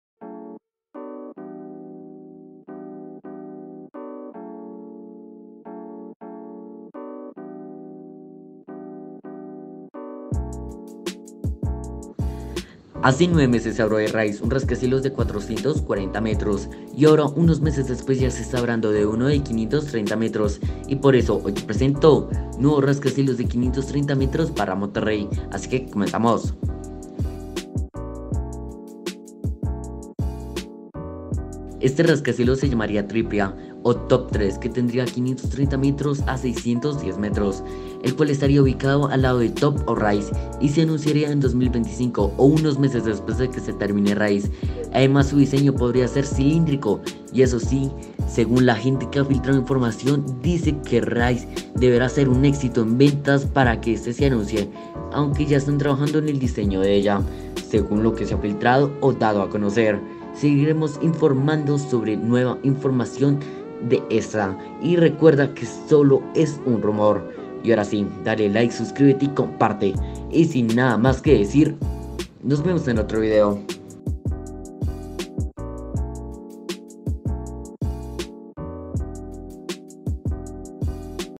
🇲🇽NUEVO RASCACIELOS DE +600 METROS TOP 3😱 / MEGAPROYECTOS
¿ se va hacer?

*para leer*

HACE UNOS 9 MESES SE HABRO DE RISE UN RASCACIELOS DE 440 METROS Y AHORA UNOS MESES DESPUES YA SE ESTA HABLANDO UNO DE 530 METROS Y POR ESO HOY TE PRESENTO NUEVO RASCACIELOS DE 530 METROS PARA MONTERREY ASI QUE COMENZAMOS
ESTE RASCACIELOS SE LLAMARIA TRIPLIA O TOP 3 QUE TENDRIA 530 METROS A 610 METROS EL CUAL ESTARIA UBICADO ALADO DE TOP O RISE Y SE ANUNCIARIA EN 2025 O UNOS MESES DESPUES DE QUE SE TERMINE RISE ADEMAS SU DISEÑO PODRIA SER CILINDRICO Y ESO SI SEGÚN LA GENTE QUE A FILTRADO LA INFORMACION DICE QUE RISE DEBERA SER UN ÉXITO EN VENTAS PARA QUE ESTE SE ANUNCIE AUNQUE YA ESTAN TRABAJANDO EN EL DISEÑO DE ELLA SEGÚN LO QUE SE A FILTRADO O DADO A CONOCER
SEGUIREMOS INFORMANDO SOBRE NUEVA INFORMACION DE ESTA Y RECUERDA QUE SOLO ES UN RUMOR

□CANALES QUE TAMBIÉN HABLAN DE PROYECTOS □

TODO LO QUE TIENES QUE SABER DE MITIKAH / MEGAPROYECTOS

Esto es MEGAPROYECTOS